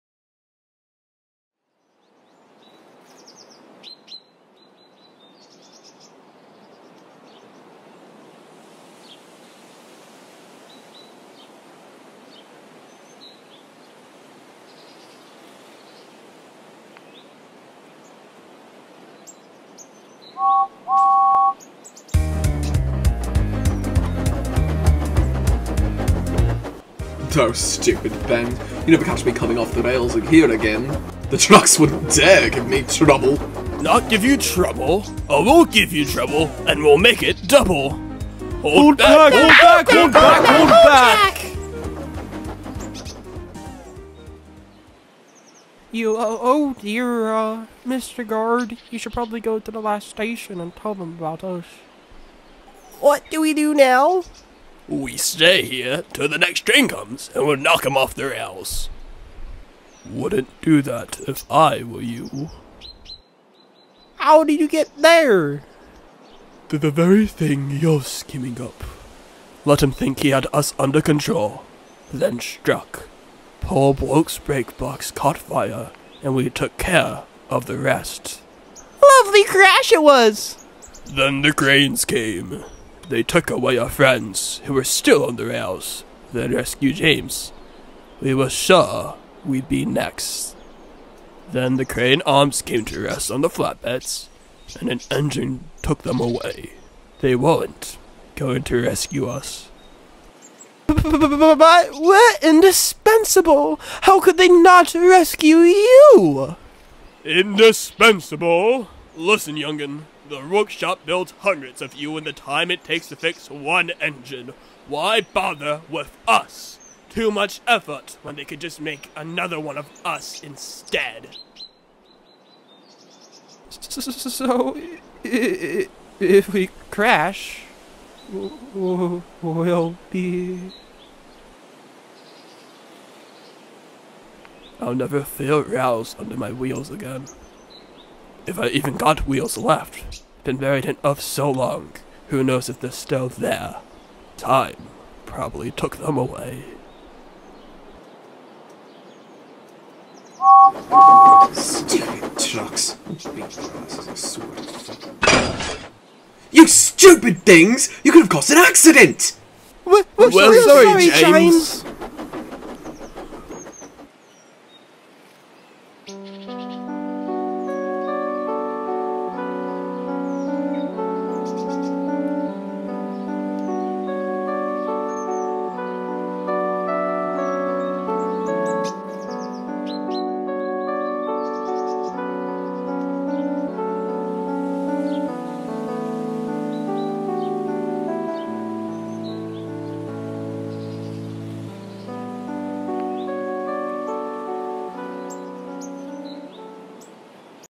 Left Behind | Yet Another Tale 9
“One evening as the sun was beginning to set, James was pulling a goods train along the mainline. He was making excellent time, and the trucks were behaving well. He felt most proud of himself . . .”
“Left Behind” is a story written by The Buried Truck
Thomas and friends, the franchise, and it’s character’s is NOT owned by me
The Sudrian Chronicles and Yet Another Tale is created by LNERLiam